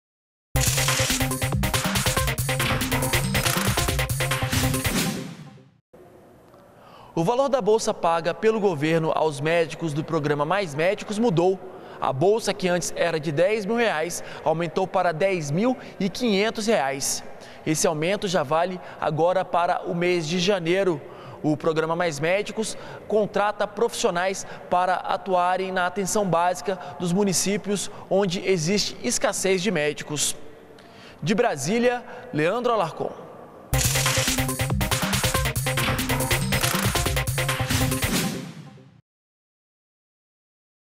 Bolsa-formação para profissionais do programa Mais Médicos é reajustada
ÚLTIMAS - 19.01.15: O Ministério da Saúde reajustou o valor da bolsa-formação para os profissionais participantes do Programa Mais Médicos. A bolsa que era paga no valor de R$10.000,00, passa a ser de R$10.513,01 a contar da competência janeiro de 2015. O Mais Médicos leva atendimento para municípios onde há falta de profissionais.